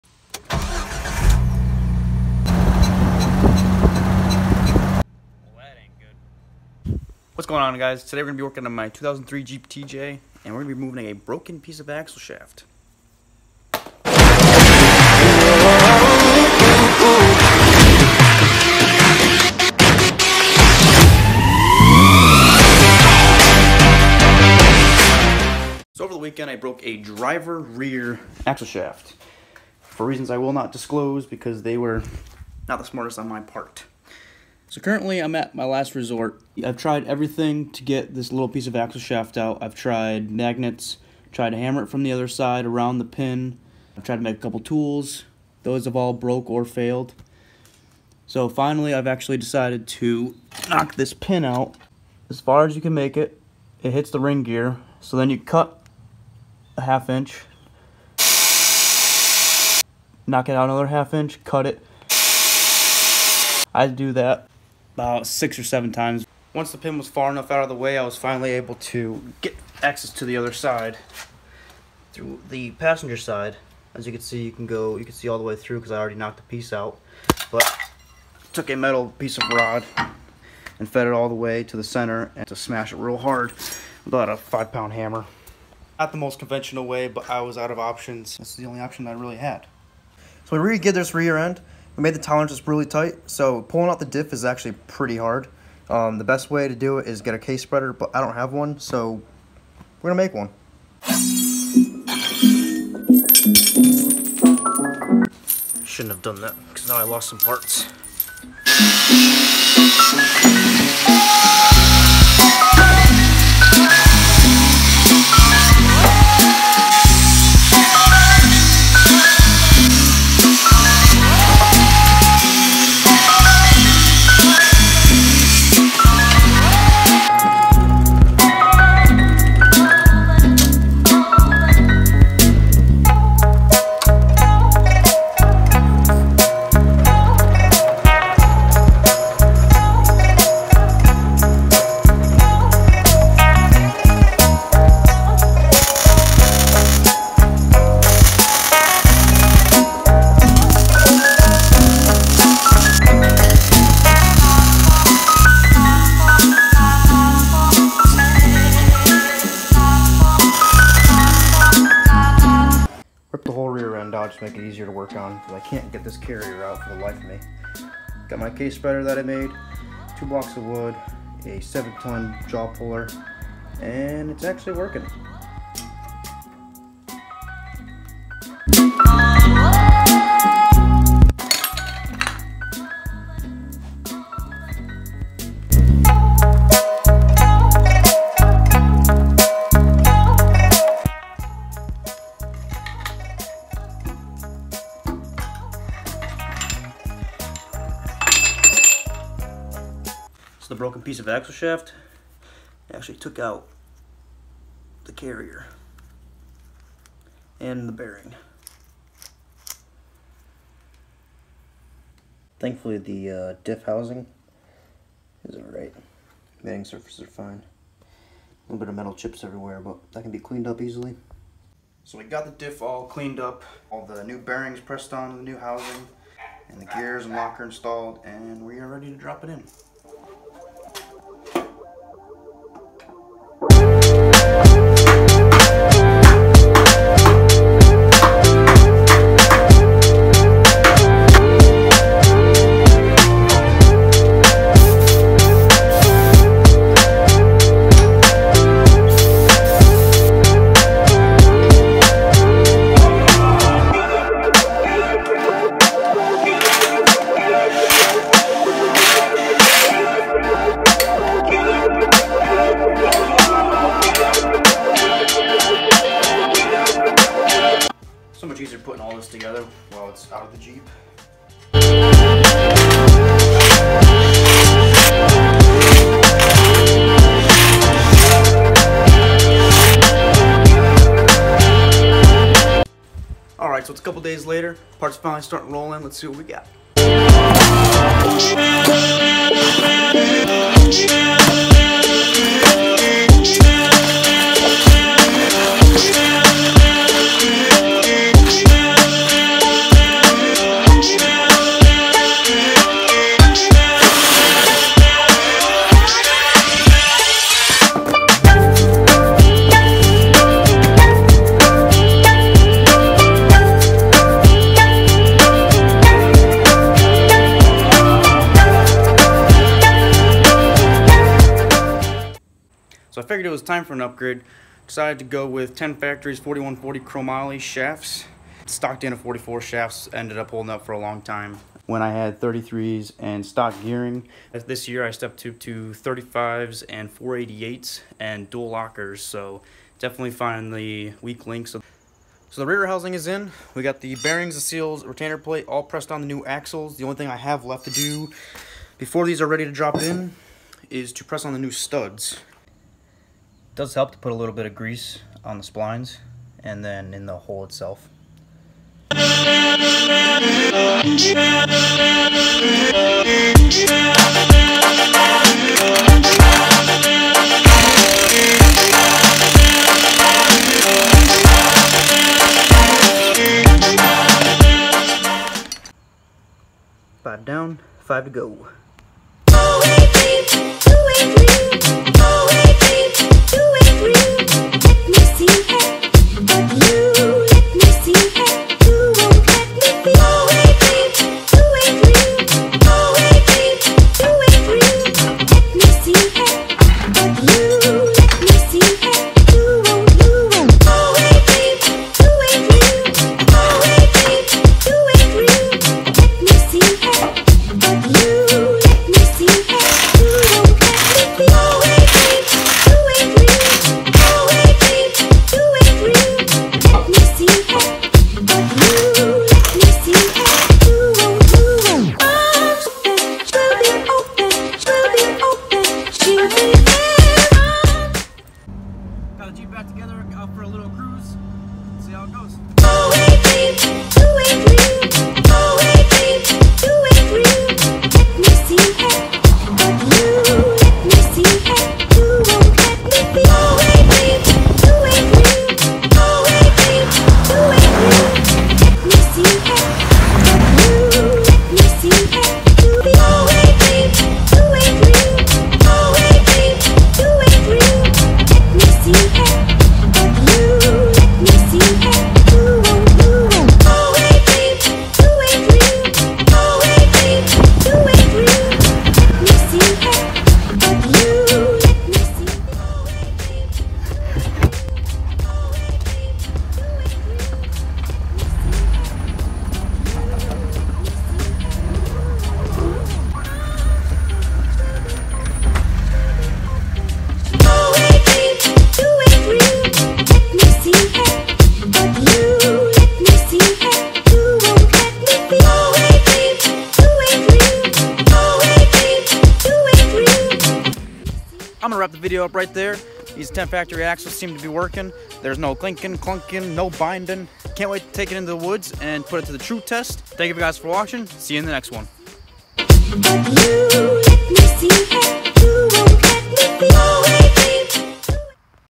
Removing a Broken Piece of Axle Shaft: Caused A lot More Damage Than Expected
Get a glimpse at how I removed a broken piece of axle shaft in the rear dana 44 of my Jeep Wrangler and end the video with a scenic cruise to test out the repairs

Musician: Not The King.👑